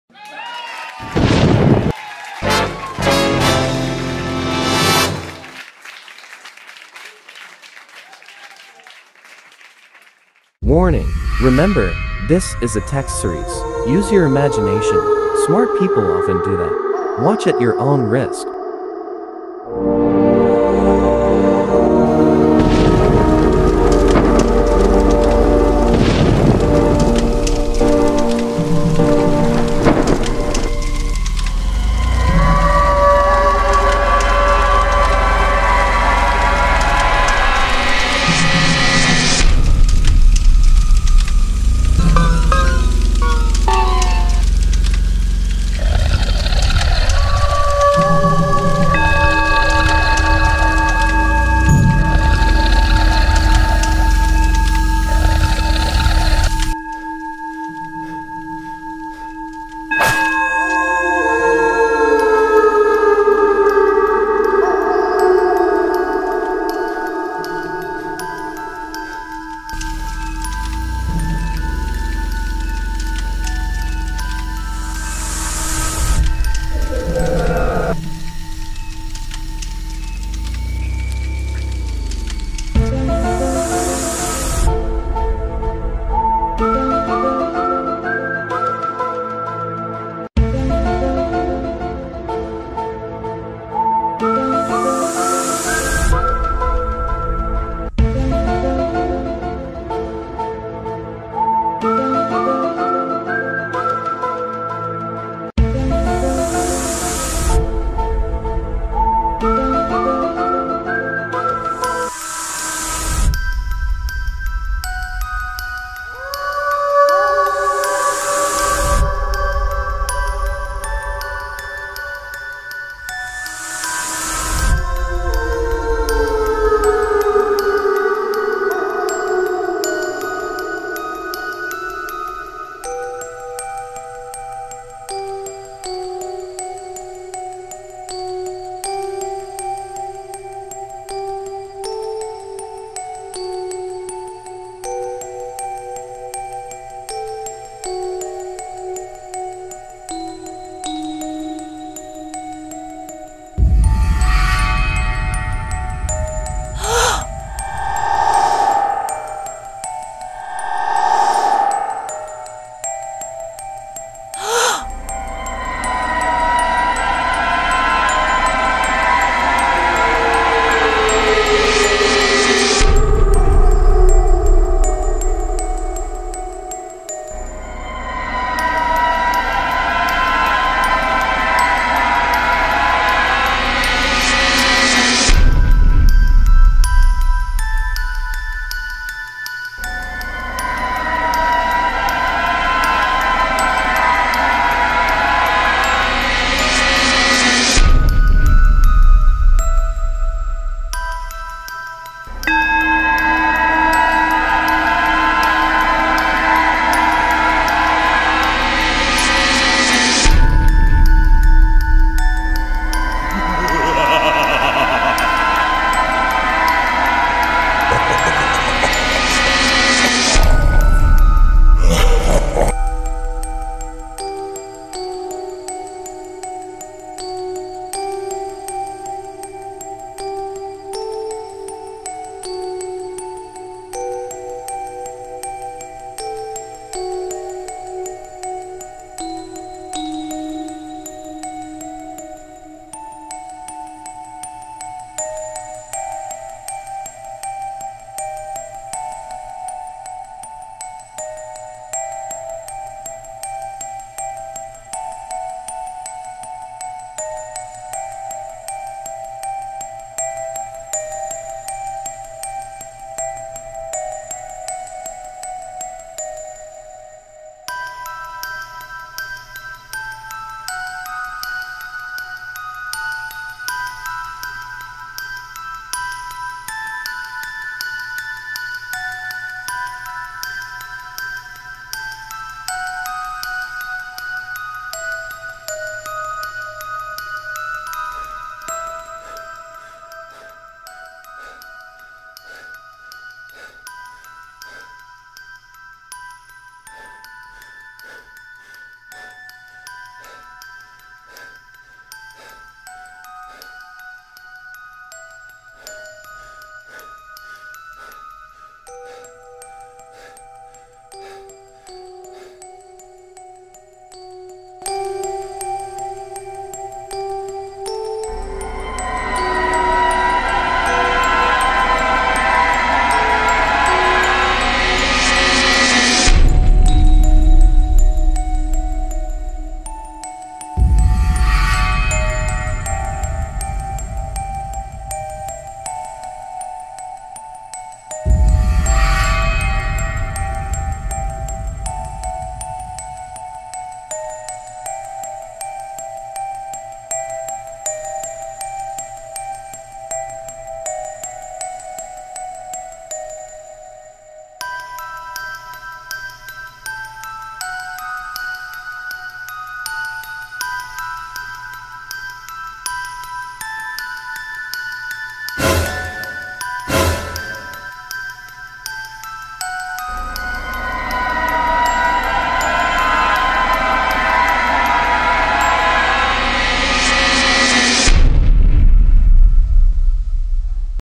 Kababalaghan Textseries : Ang Storya ni Mang Canor
A true urban legend story by a man. This is a short video only of a man's creepy experiences. Hope you'll enjoy watching this.

Please follow the fb page of humor